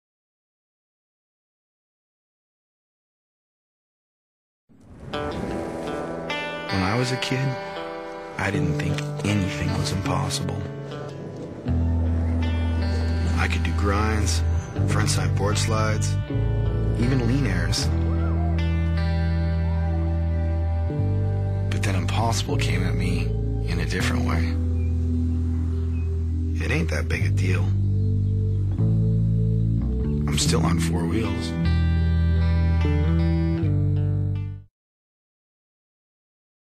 STACY KOHUT PROMO 2004(1)
Stacy Kohut adidas commercial 2004.Footage consists of "extreme" wheelchair skatepark.

Stacy Kohut's FOUR LIFE.